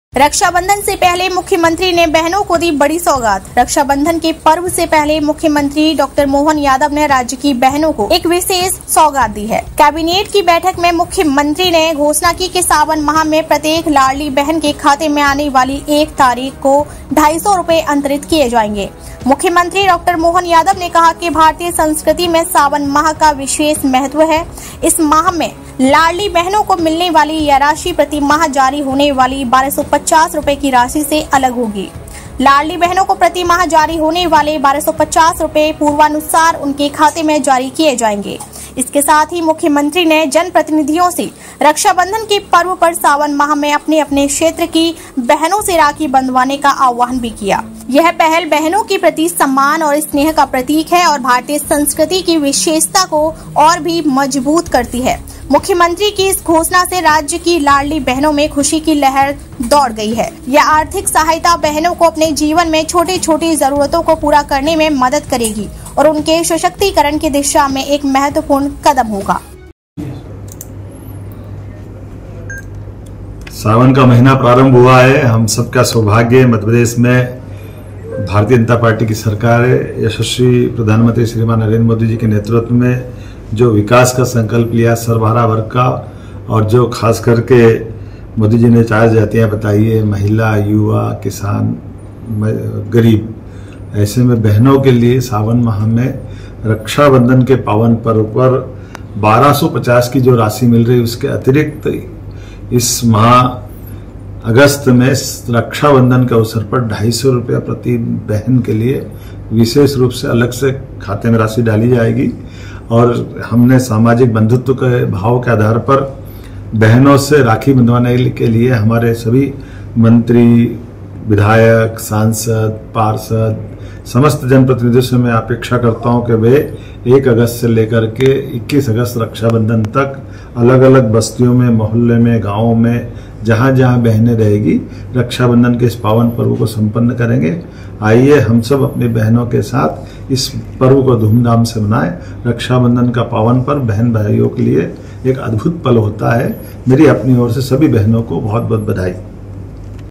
Laldli Bhena Yojna : रक्षाबंधन से पहले Madhya Pradesh CM Mohan Yadav ने बहनों को दी बड़ी सौगात
Laldli Bhena Yojna Madhya Pradesh : रक्षाबंधन से पहले Madhya Pradesh CM Mohan Yadav ने बहनों को दी बड़ी सौगात

 रक्षाबंधन के पर्व से पहले मुख्यमंत्री डॉ. मोहन यादव ने राज्य की बहनों को एक विशेष सौगात दी है। कैबिनेट की बैठक में मुख्यमंत्री ने घोषणा की कि सावन माह में प्रत्येक लाडली बहन के खाते में आने वाली एक तारीख को 250 रुपए अंतरित किए जाएंगे।